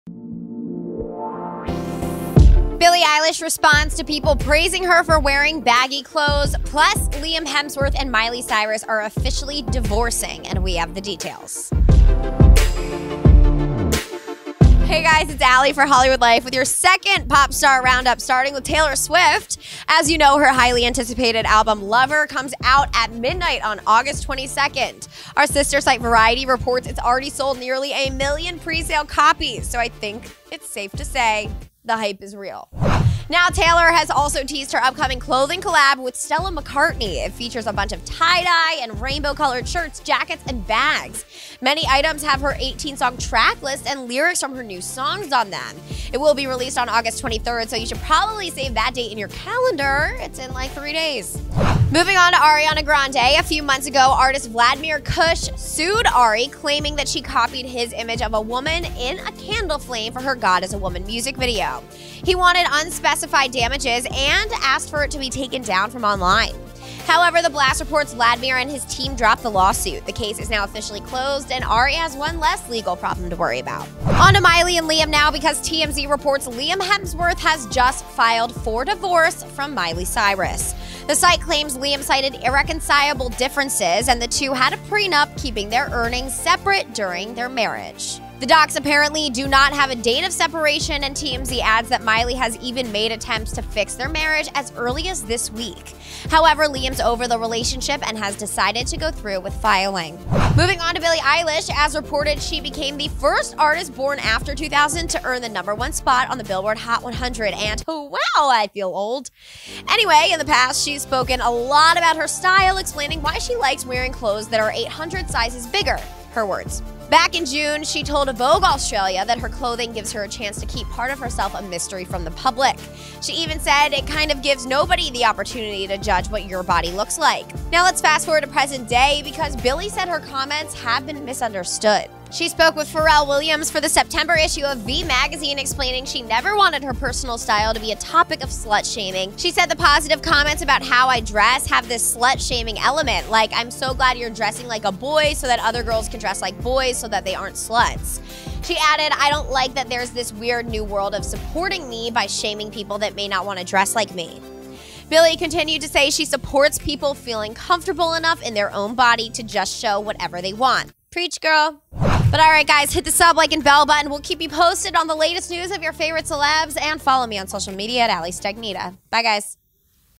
Miley Cyrus Reaction To Liam Hemsworth Divorce Filing
Billie Eilish claps back at people praising her for wearing baggy clothes. Plus – Liam Hemsworth files for divorce from Miley Cyrus.

“You don’t understand what it’s like,” Liam Hemsworth, 29, told Daily Mail Australia when he was asked about his breakup with wife Miley Cyrus, 26, during an outing in Byron Bay, Australia, on Aug. 12. “I don’t want to talk about it, mate.” This was Liam’s first appearance since the shocking breakup and his first words about the split. He joined his older brother, Chris Hemsworth, 36, on a trip to town with the Thor star’s kids: India, 7, and twins Tristan and Sasha, 5. Liam went with Chris and his kids to a frozen yogurt store. The hunky actor, wearing a beige hoodie and black jeans, looked visibly upset while walking around town.

Starring @alistagnitta
Producing @BraunLA

ABOUT HOLLYWOODLIFE